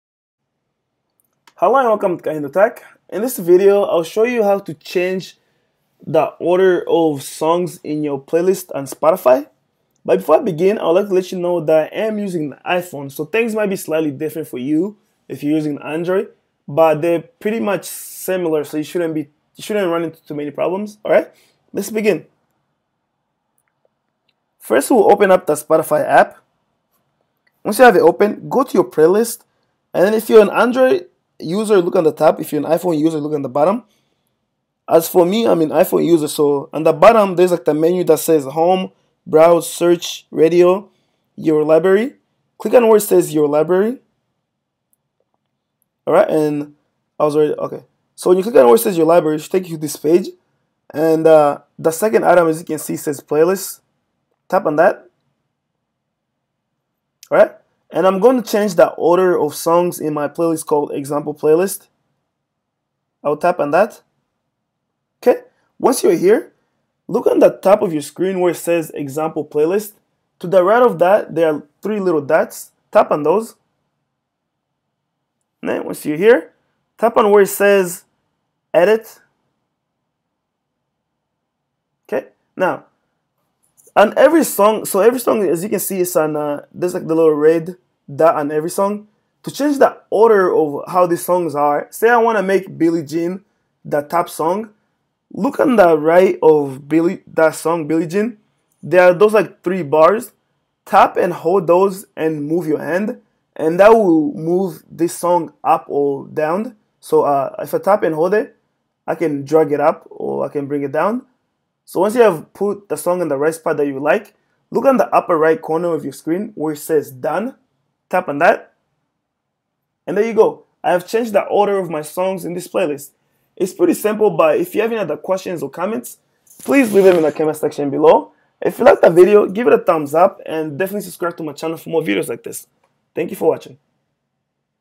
How To Change Order Of Songs In A Playlist Spotify
This video shows How To Change Order Of Songs In A Playlist Spotify.

►► FREE Amazon Prime 30-Day Trial